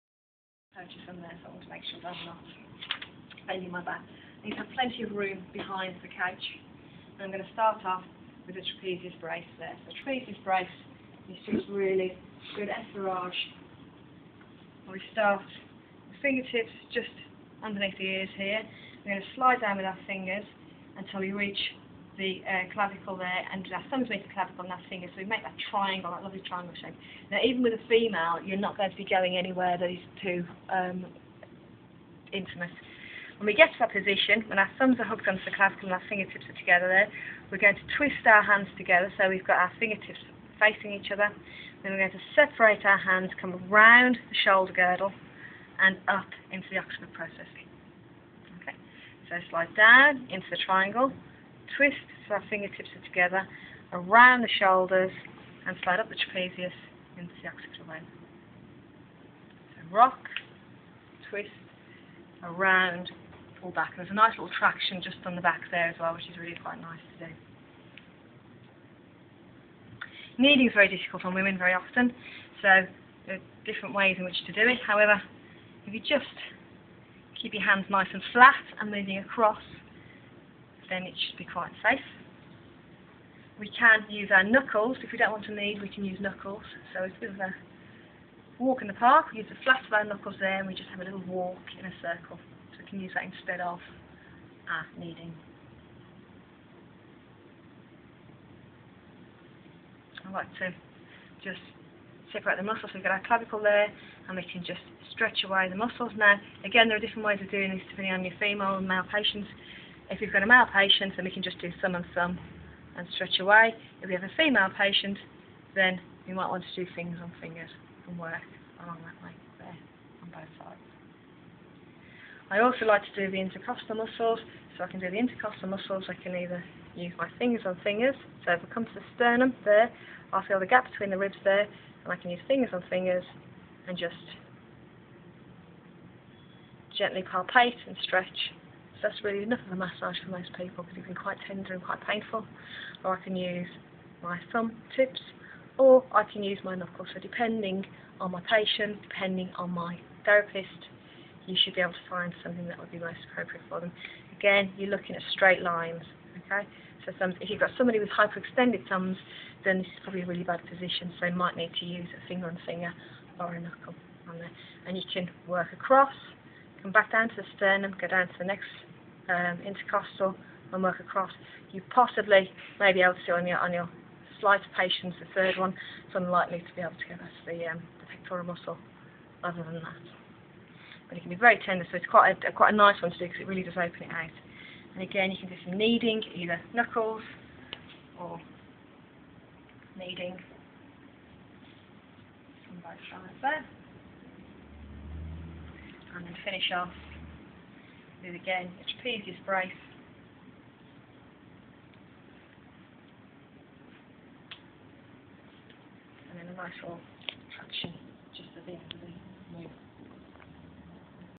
General Sports Massage Techniques. Supine. Chest..3gp
Basic sports massage techniques for the chest region.